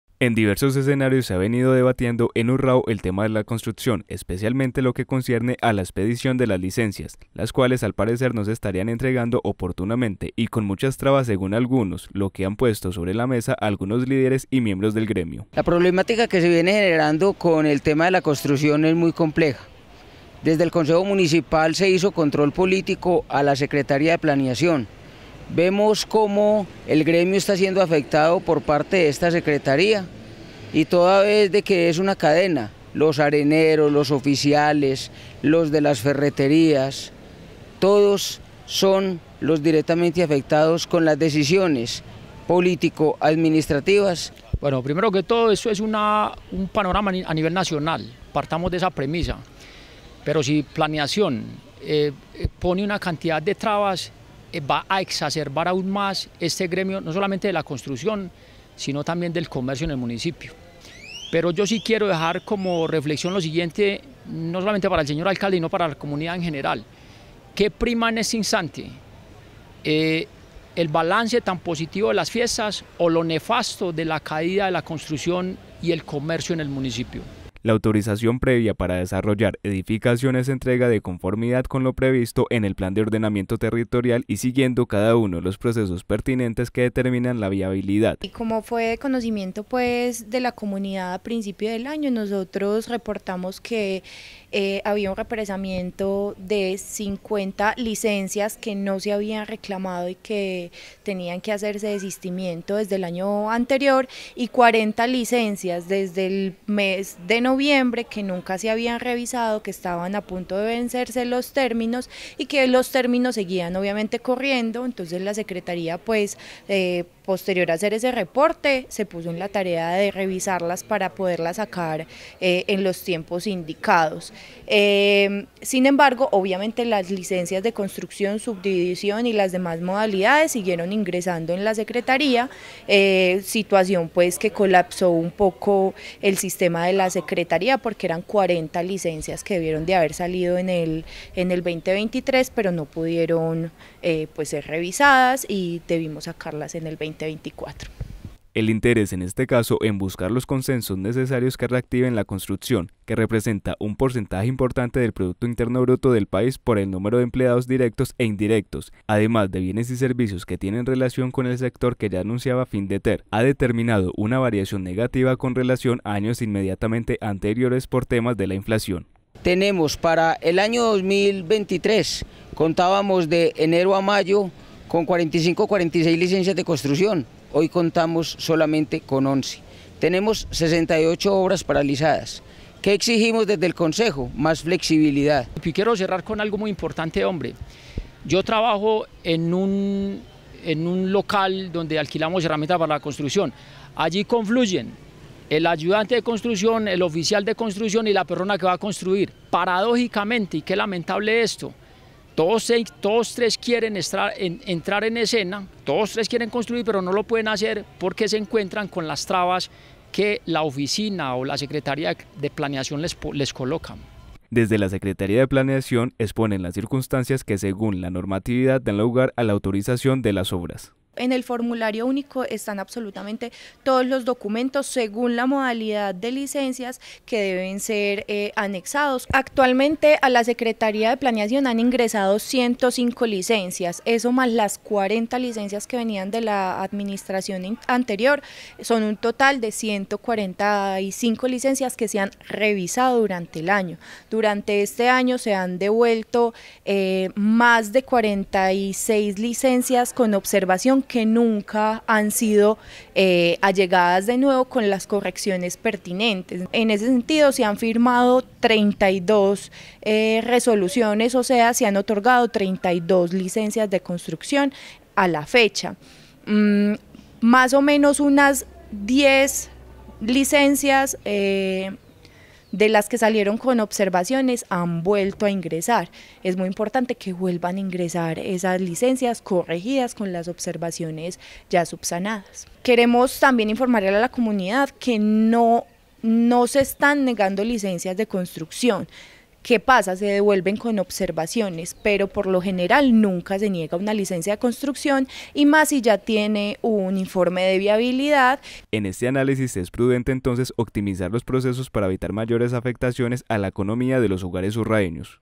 Inconformidades con licencias de construcción
Analizaremos cuál es el proceso que se debe seguir para adquirir una licencia de construcción, pero también la inconformidades de una parte del gremio que aduce hay muchas trabas para que se entregue las autorizaciones que permiten iniciar las obras, por lo que se estarían viendo afectadas decenas de familias que viven de la construcción.